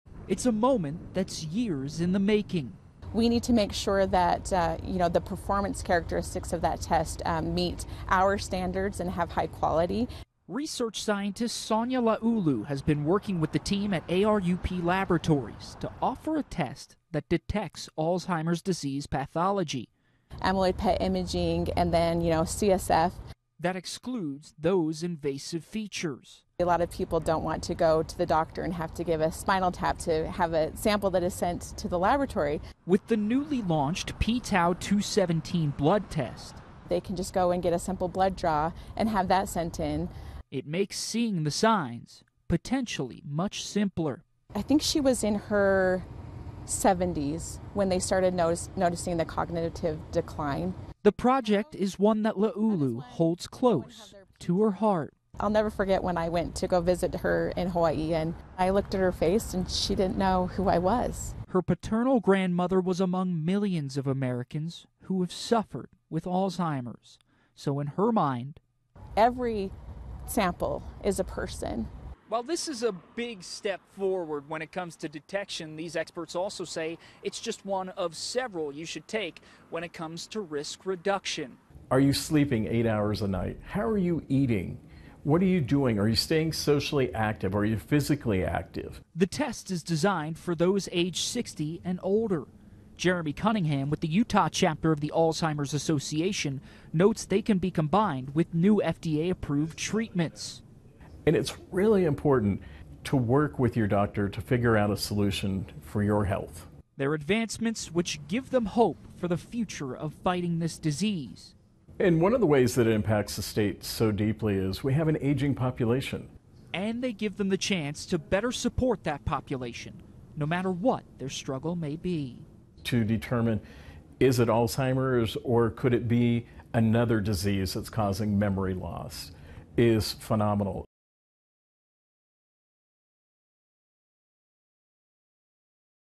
New, 'phenomenal' test for Alzheimer's is now in Utah labs
DETECTING ALZHEIMER'S- A new blood test used to detect the pathology of Alzheimer’s Disease has just launched in Utah, and local experts are calling it revolutionary.

The pTau 217 Blood Test is something that’s been worked on for years at ARUP Laboratories on the University of Utah’s campus.